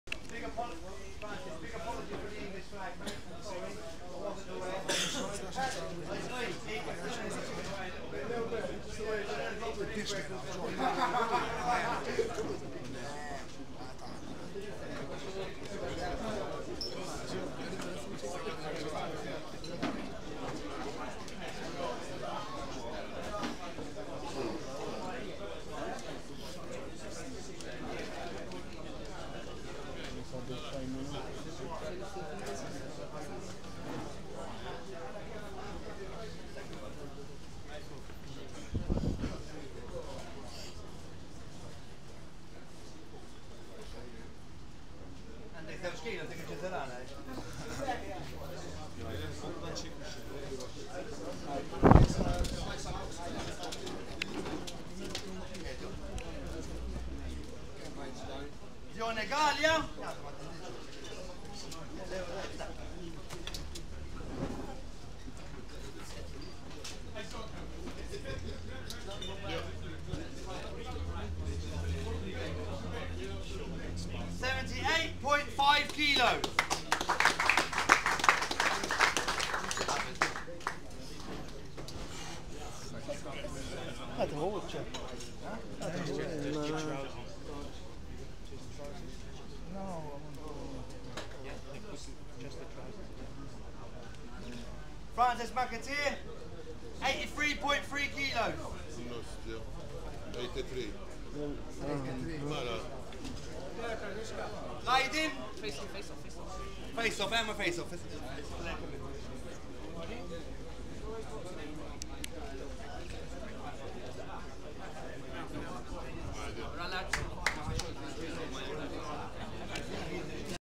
Dione 'maroushka' Galea vs Fran 'bambam' McAteer (weigh-in)
Boxing weigh-in at Sliema Malta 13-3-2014, Galea from Malta 78.5kg & McAteer from Ireland. 83.3kg.